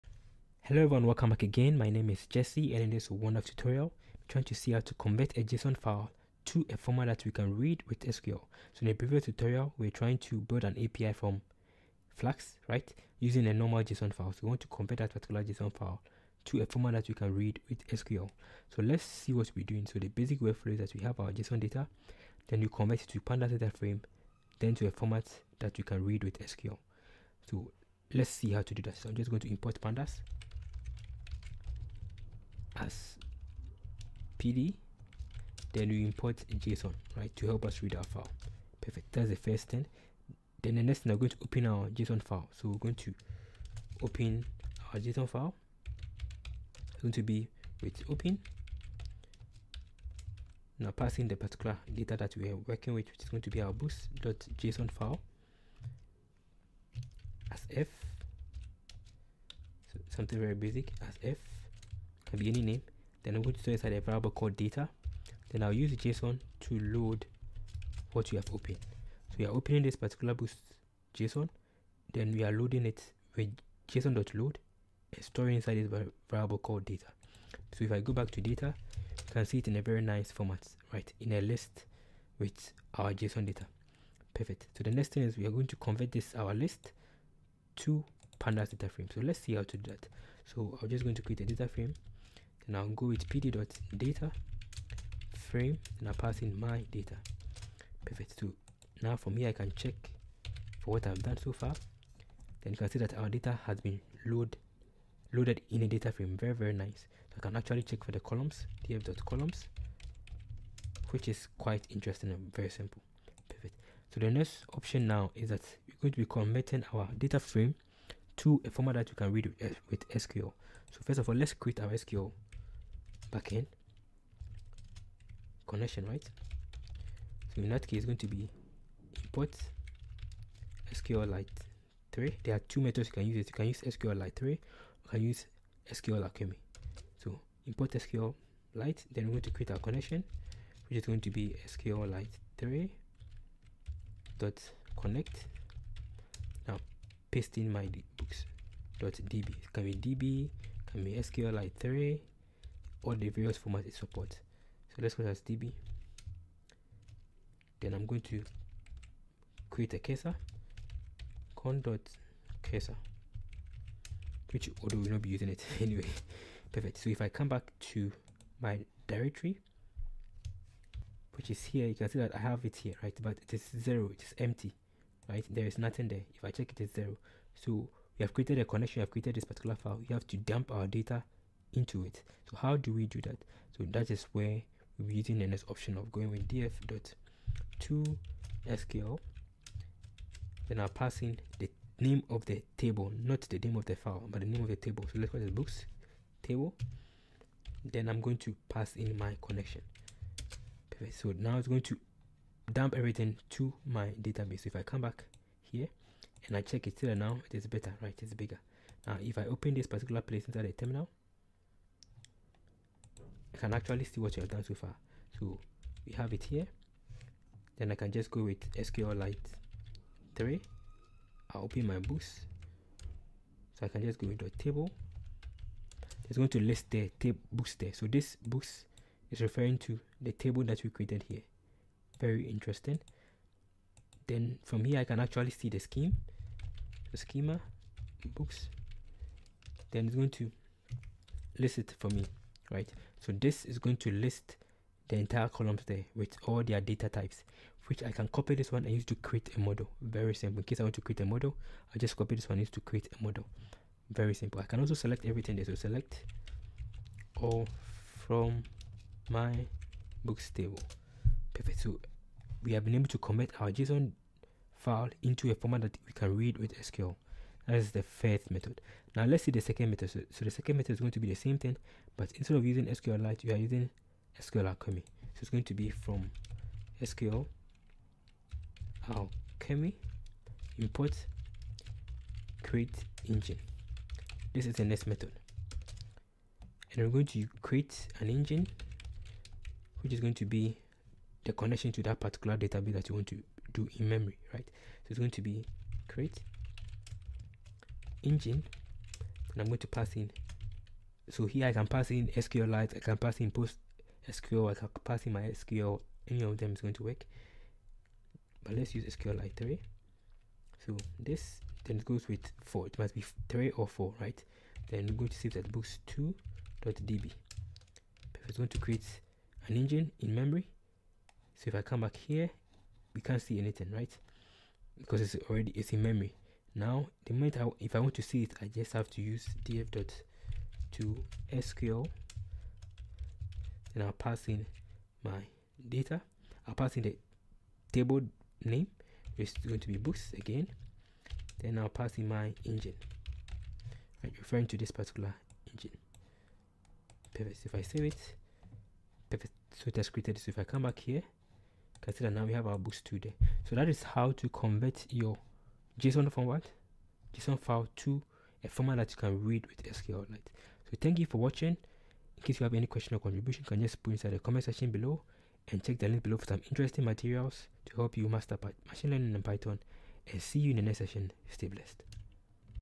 How To Convert JSON to SQL format
In this tutorial we will see how to convert JSON to a format we can read with SQL. We will be using two methods for this task.

J-Secur1ty
Jesus Saves @ JCharisTech